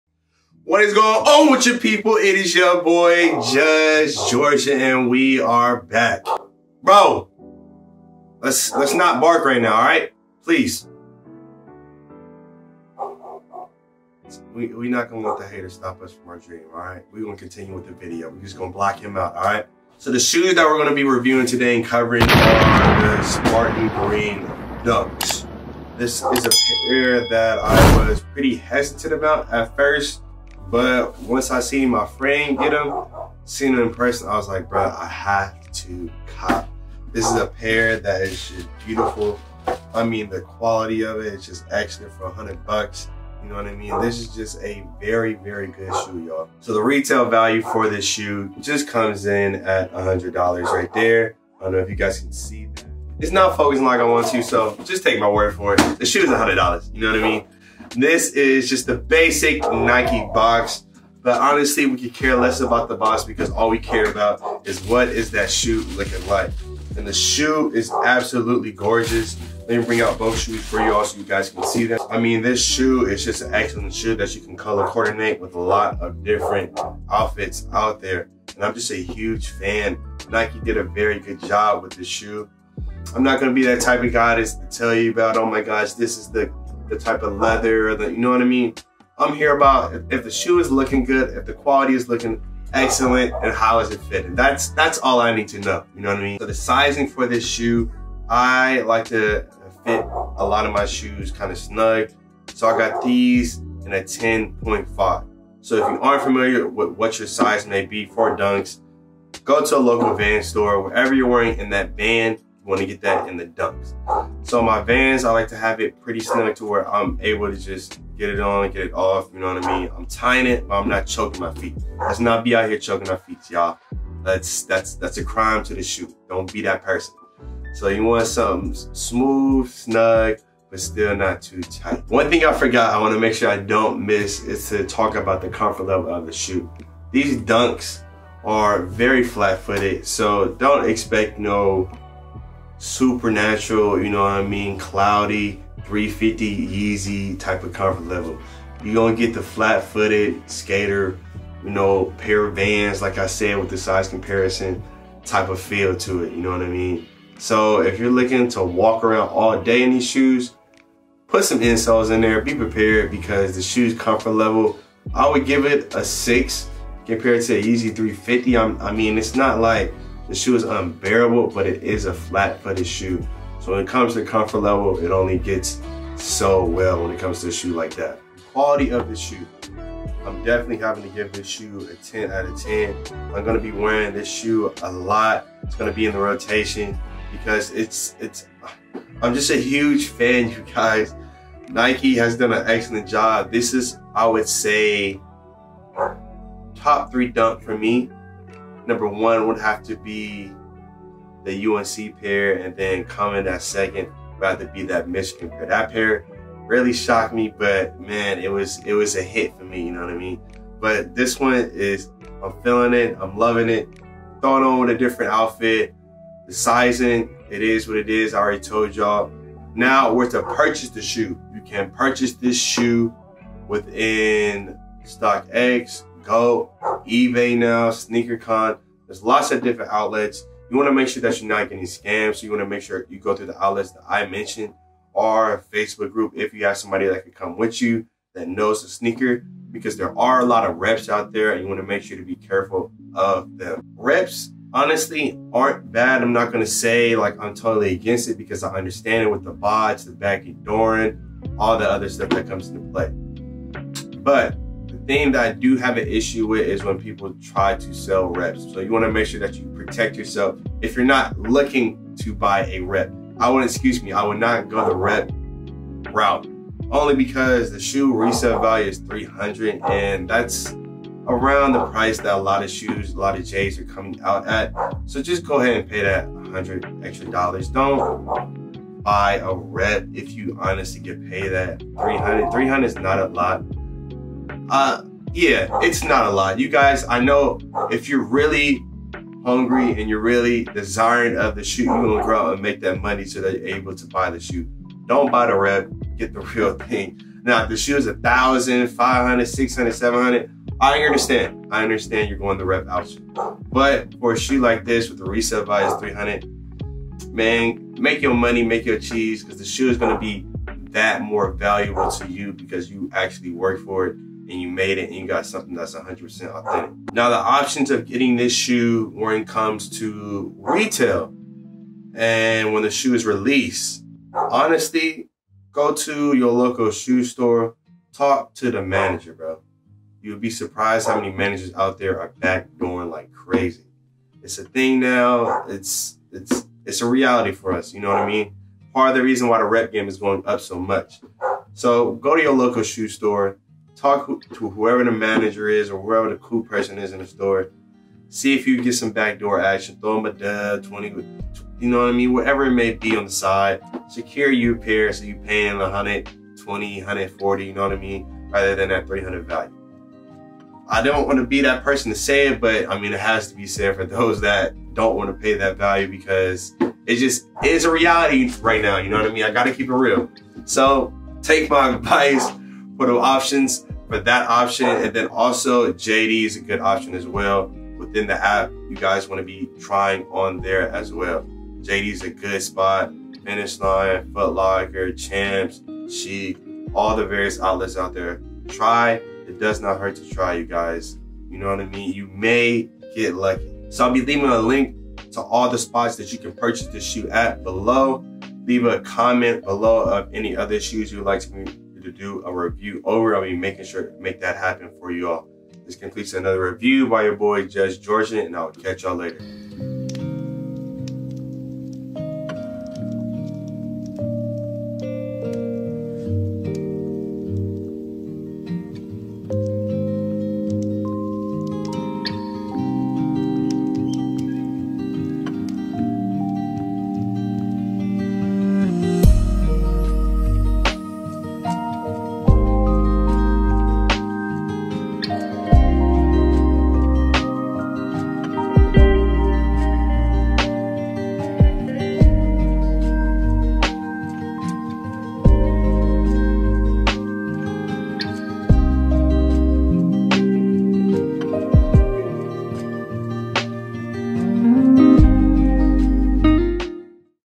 Spartan Green Dunks | Sizing + On Foot Review + Info On How Not To Lose On Shoe Drops
My Height Is: 6'0ft  My Weight: 195lbs
Size I Purchased In These Items Was A 10.5
Link To Shoe Sites